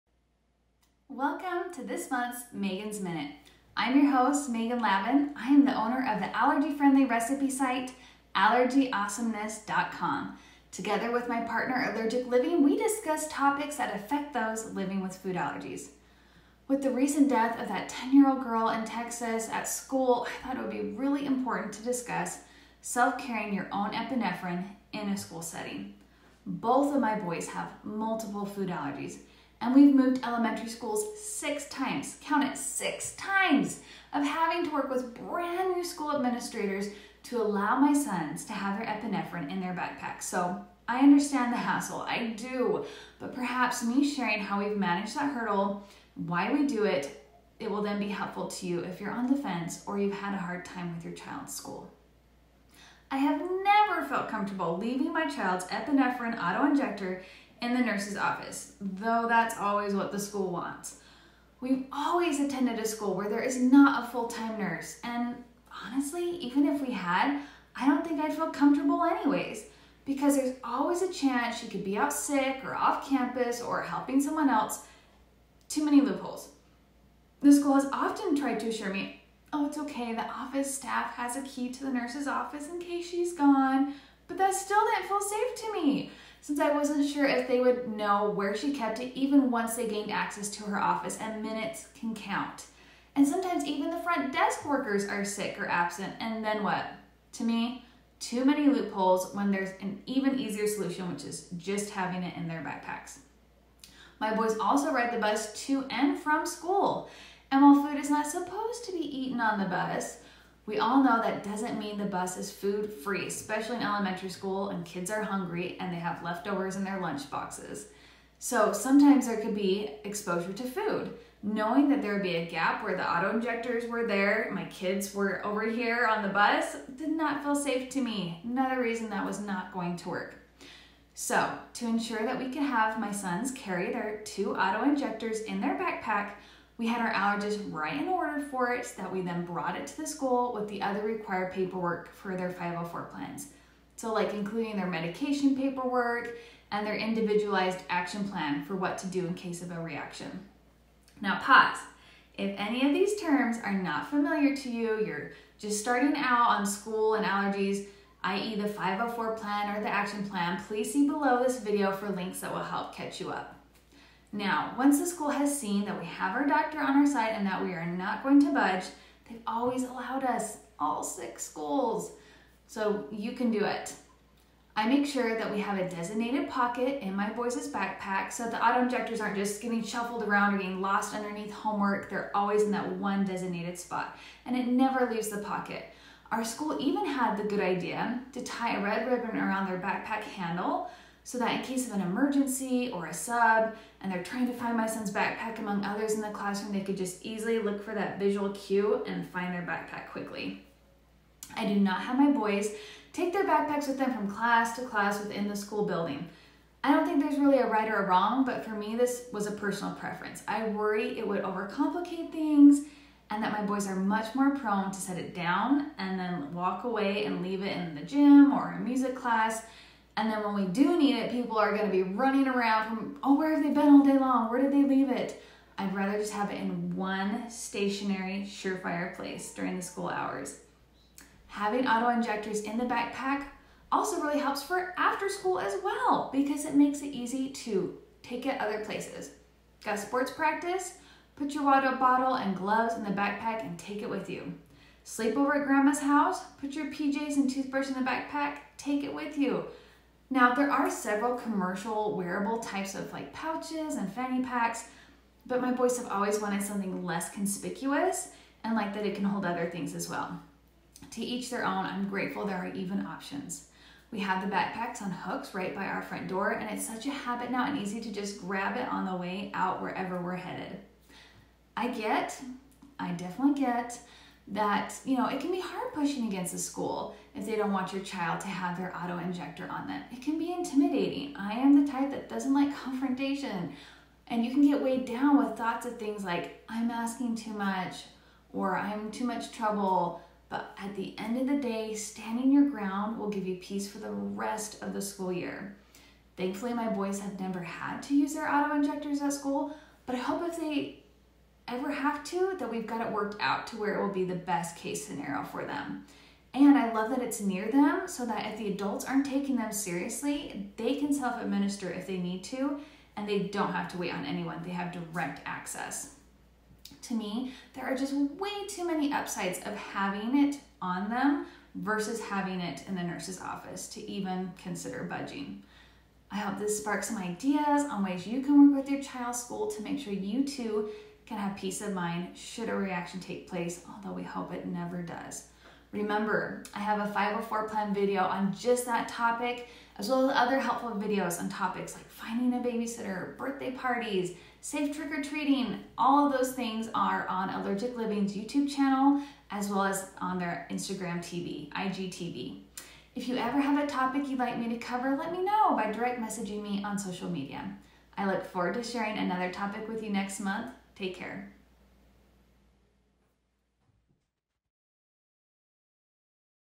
Kids Self-Carrying Epinephrine | Megan’s Minute
The school wants all meds locked up with the school nurse. As an allergy parent, you want your allergic child to self-carry his/her epinephrine for quick emergency access. What to do? Contributor Megan Lavin explains her process to win the right to carry epi.

In the video, Megan covers:

• How she's won the right to self-carry epi at 6(!) schools.
• Why she views backpacks as "the place" for auto-injectors.
• Where the backpacks stay at school; why they work for the bus.
• Getting an allergist's note that verifies the need to self-carry.
• Including the self-carry issue in 504 plan discussions.
• Creating a backpack-carrying habit with kids.

On this important safety protocol, Megan finds that "standing your ground will bring you peace for the rest of the school year."